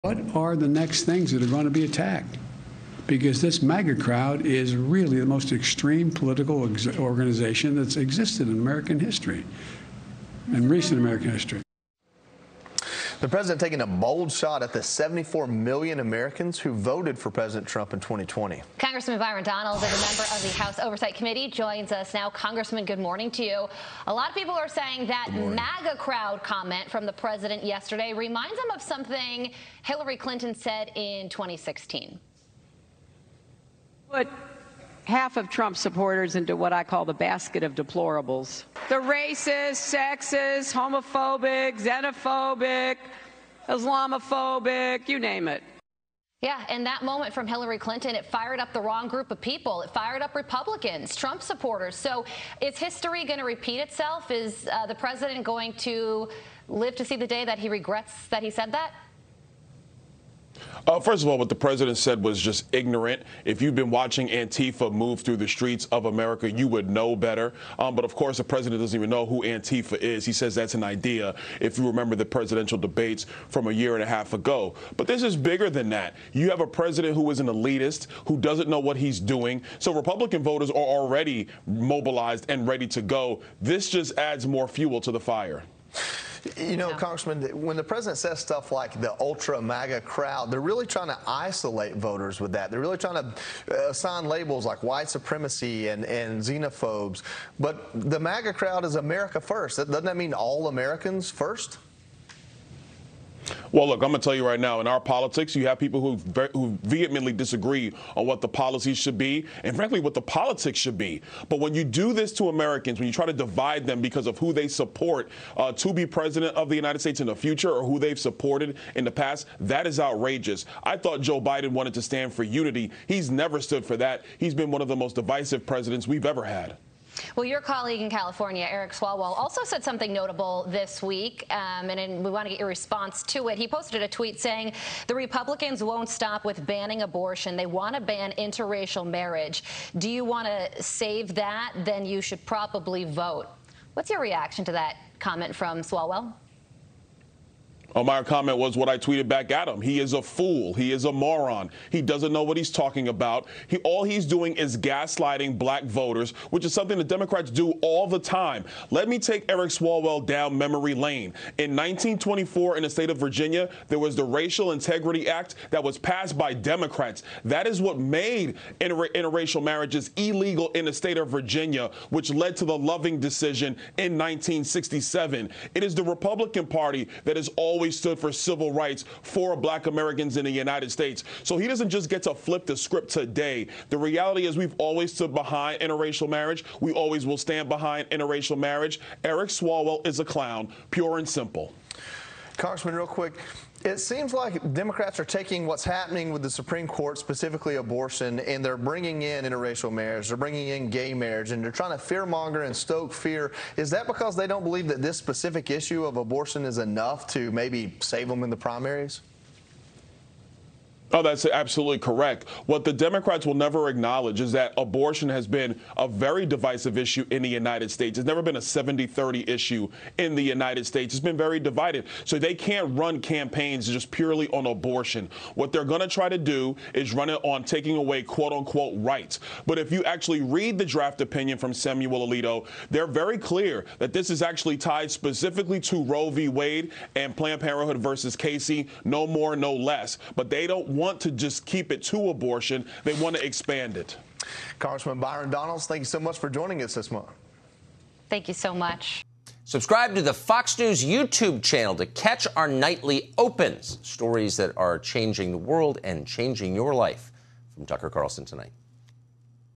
Swalwell blasted for claiming GOP wants to ban interracial marriage
Rep. Byron Donalds, R-Fla., slams Biden for accusing the MAGA movement of being an extreme group and responds to Eric Swalwell's tweet accusing Republicans of wanting to ban interracial marriage.
BYRON DONALDS: In our politics, you have people who very vehemently disagree on what the policy should be, and frankly, what the politics should be. But when you do this to Americans, when you try to divide them because of who they support to be President of the United States in the future or who they've supported in the past, that is outrageous. I thought Joe Biden wanted to stand for unity. He's never stood for that. He's been one of the most divisive presidents we've ever had.